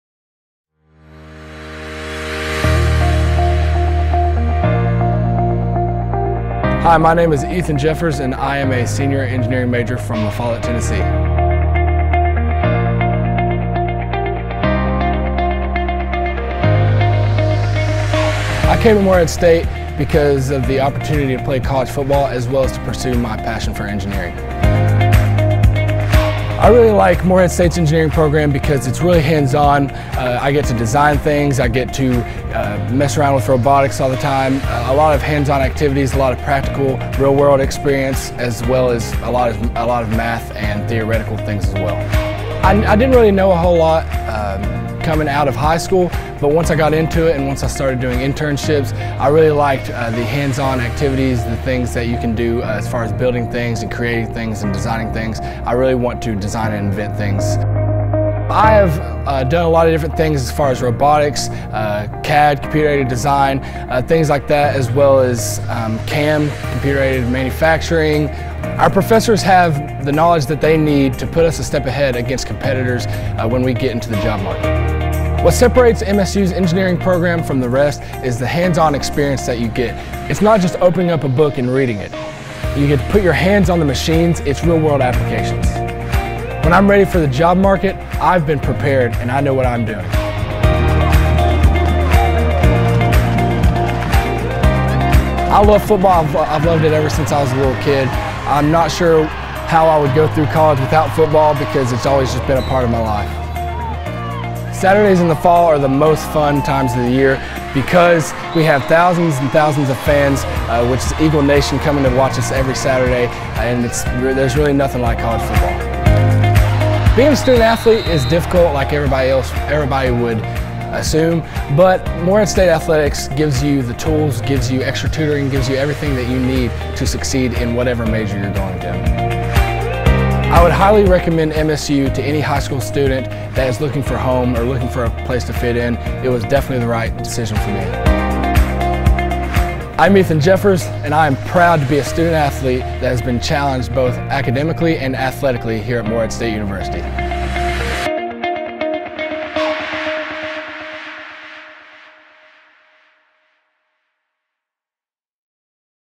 Ethan Jeffers - Engineering & Technology
Ethan Jeffers is a senior student/athlete at Morehead State University.  He's an Engineering major who will graduate in May 2018.